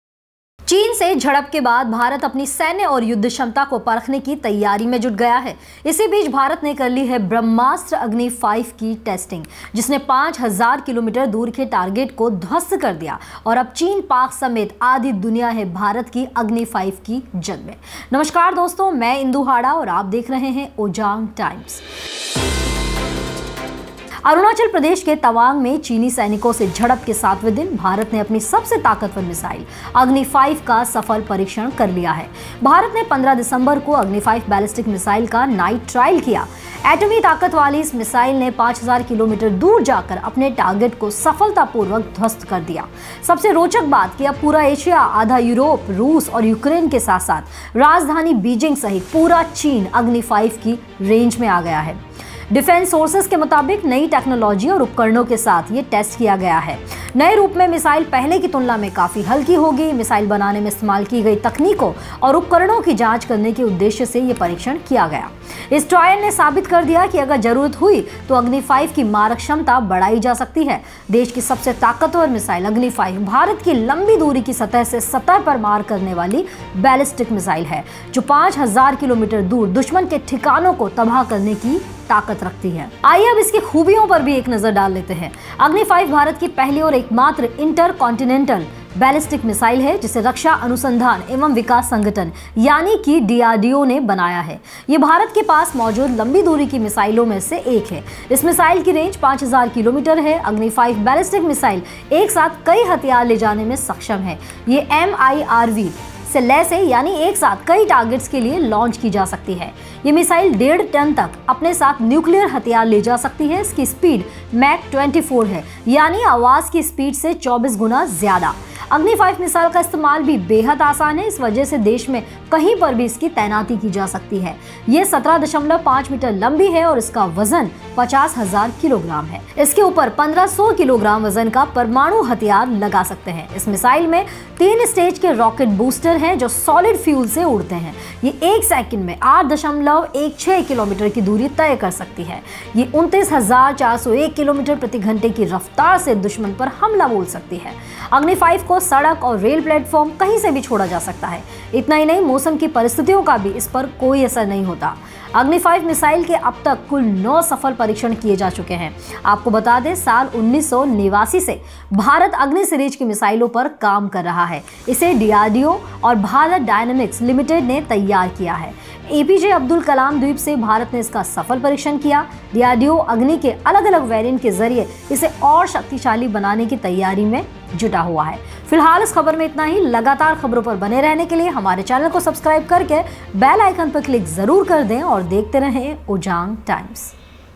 India tests Agni V missile: भारत की सबसे ताकतवर मिसाइल अग्नि 5
चीन से झड़प के बाद भारत अपनी सैन्य और युद्ध क्षमता को परखने की तैयारी में जुट गया है। इसी बीच भारत ने कर ली है ब्रह्मास्त्र अग्नि-5 की टेस्टिंग, जिसने 5 हजार KM दूर के टारगेट को ध्वस्त कर दिया और अब चीन-PAK समेत आधी दुनिया है भारत की अग्नि -5 की जद में।
अरुणाचल प्रदेश के तवांग में चीनी सैनिकों से झड़प के 7वें दिन भारत ने अपनी सबसे ताकतवर मिसाइल अग्नि-5 का सफल परीक्षण कर लिया है। भारत ने 15 दिसम्बर को अग्नि-5 बैलिस्टिक मिसाइल का नाइट ट्रायल किया। एटमी ताकत वाली इस मिसाइल ने 5 हजार किलोमीटर दूर जाकर अपने टारगेट को सफलतापूर्वक ध्वस्त कर दिया। सबसे रोचक बात, की अब पूरा एशिया,आधा यूरोप, रूस और यूक्रेन के साथ-साथ राजधानी बीजिंग सहित पूरा चीन अग्नि-5 की रेंज में आ गया है।

Follow us for News, Sports Coverage, Entertainment, Bollywood, Tech, Auto & more!